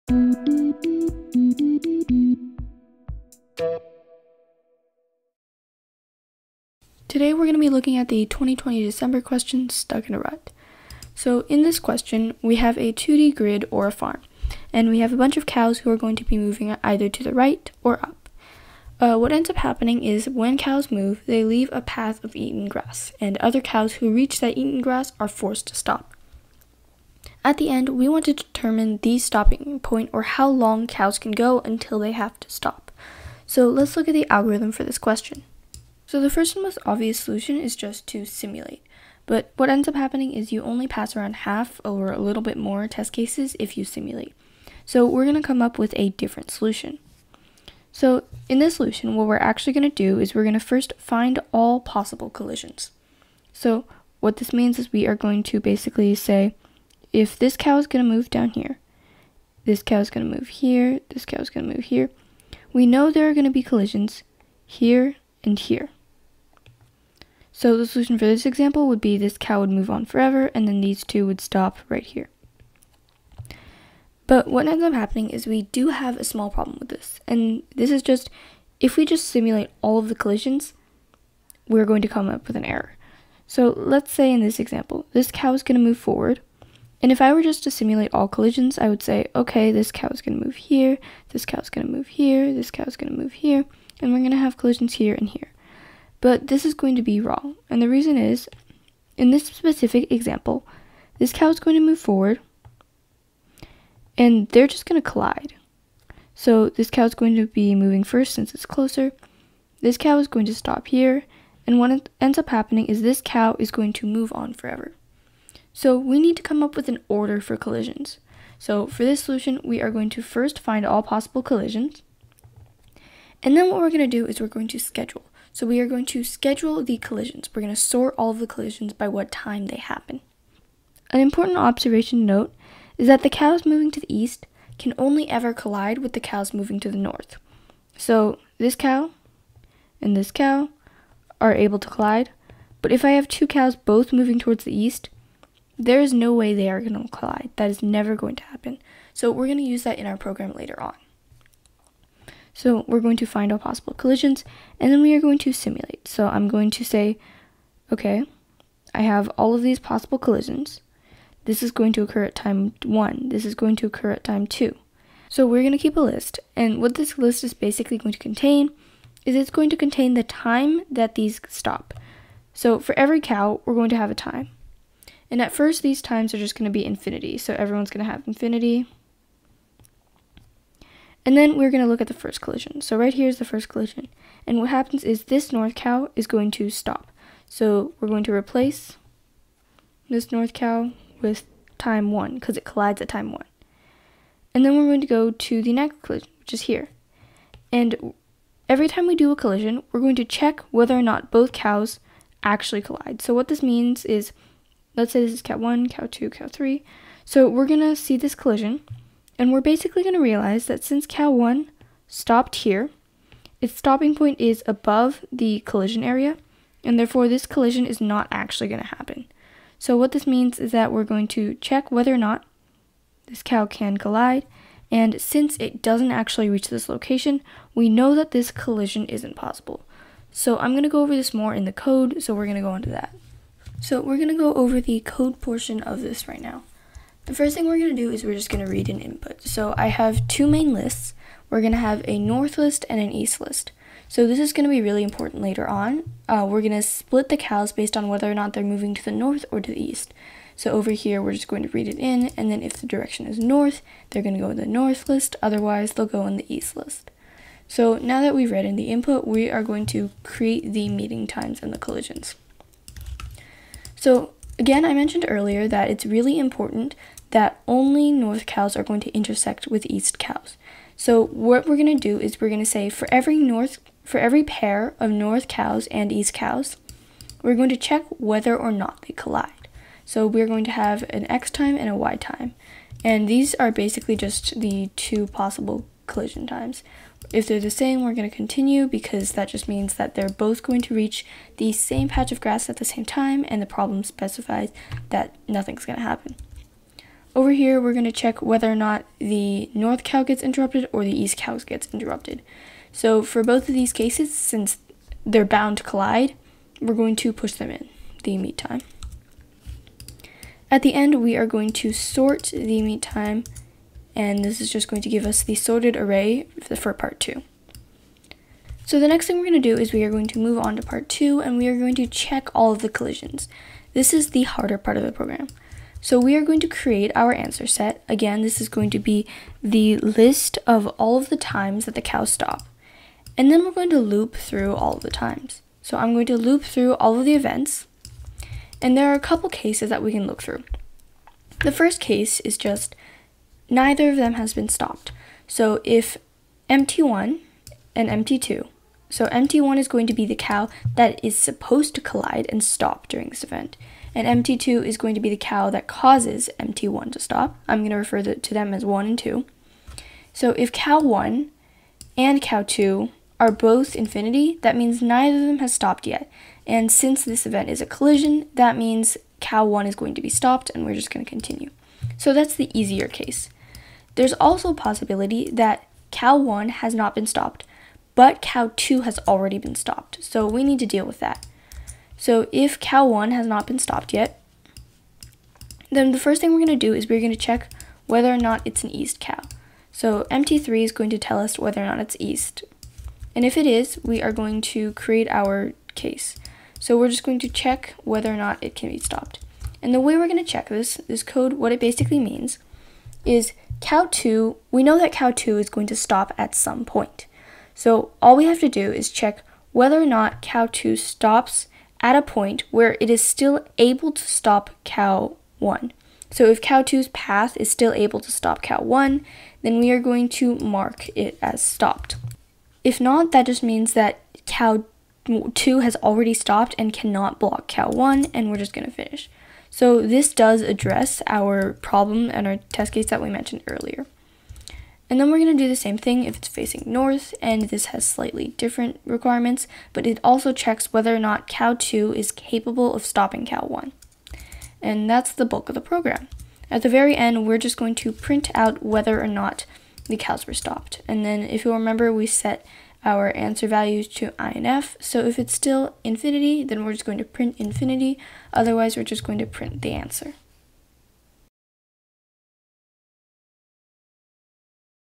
USACO 2020 Bronze December Q3: Stuck in a Rut
Solution for the USACO 2020 Bronze December Question 3, Stuck in a Rut, from starcoder.org